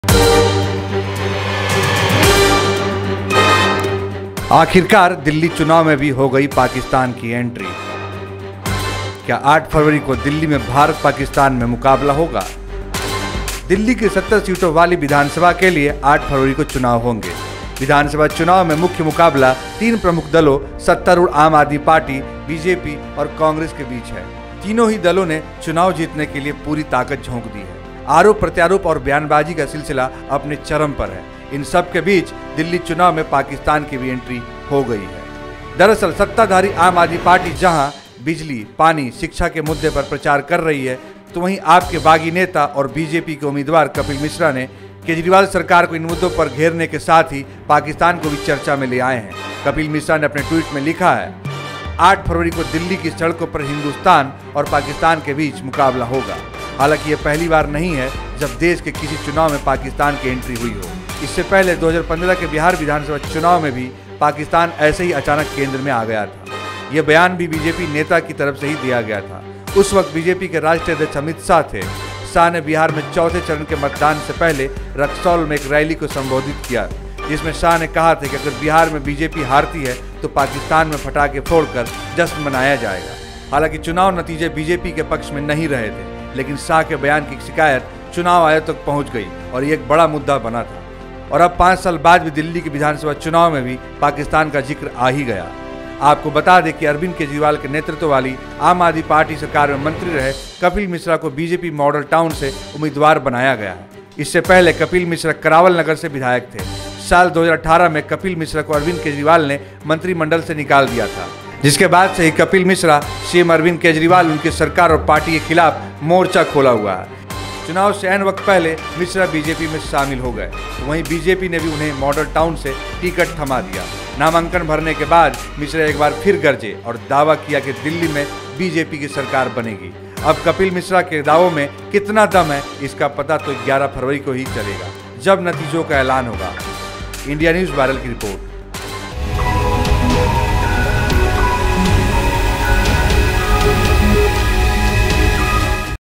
क्या 8 फरवरी को दिल्ली में भारत पाकिस्तान में मुकाबला होगा,INDIA NEWS VIRAL
आखिरकार दिल्ली चुनाव में भी हो गई पाकिस्तान की एंट्री ! क्या 8 फरवरी को दिल्ली में भारत-पाकिस्तान में मुकाबला होगा ?  जानिए रिपोर्ट में